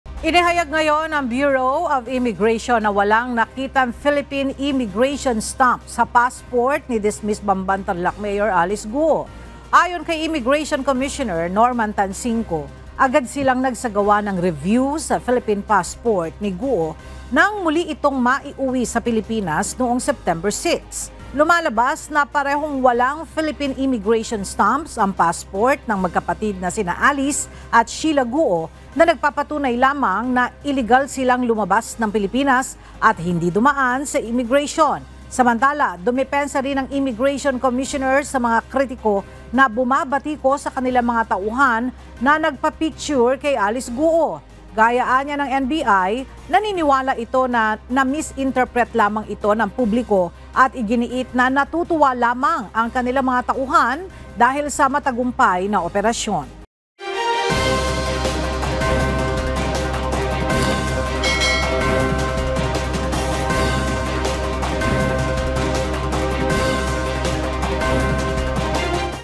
Alice Guo, hindi dumaan sa PH immigration—Bureau of Immigration
Inihayag ngayon ng Bureau of Immigration na walang nakitang Philippine immigration stamps sa passport ni dismissed Bamban Tarlac Mayor Alice Guo.

Ayon kay Immigration Commissioner Norman Tansingco agad silang nagsagawa ng review sa Philippine passport ni Guo nang muli itong maiuwi sa Pilipinas noong September 6.